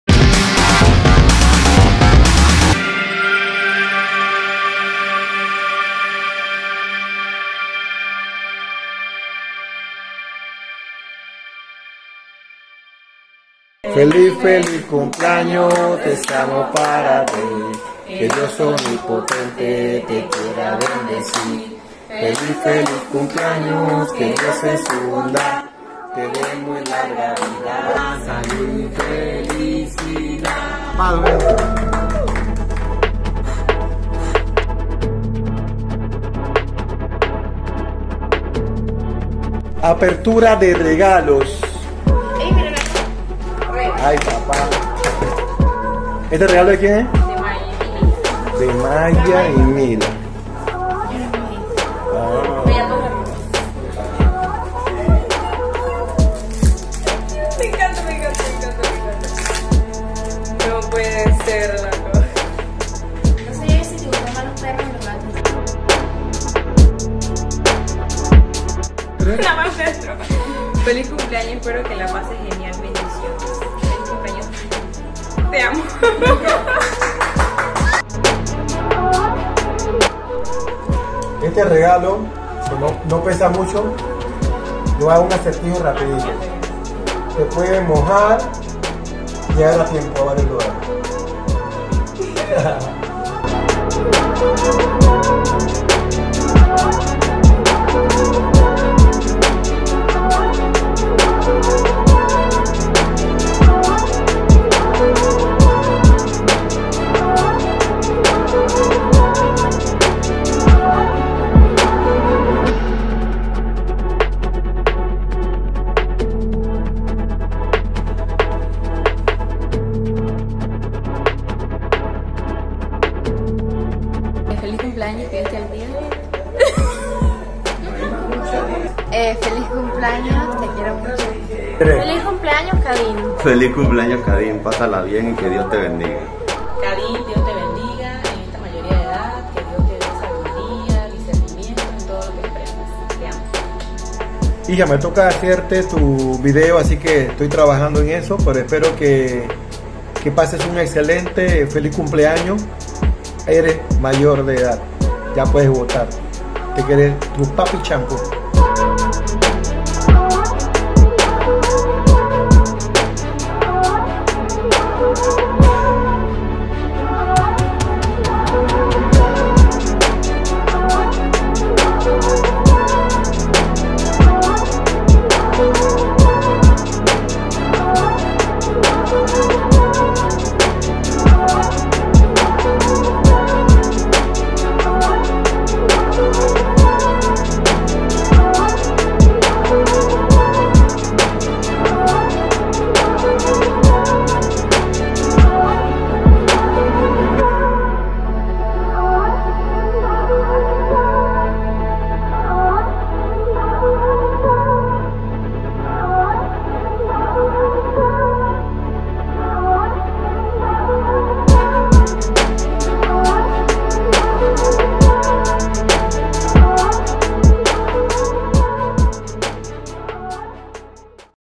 KHADINE STEPHANIE CUMPLE 18 AÑOS
Nuestra querida hija Khadine cumple su mayoría de edad. Tener 18 años en la República de Panamá es una gran responsabilidad y sabemos que en esta nueva etapa, sabrás discernir las acciones y decisiones en tu vida universitaria.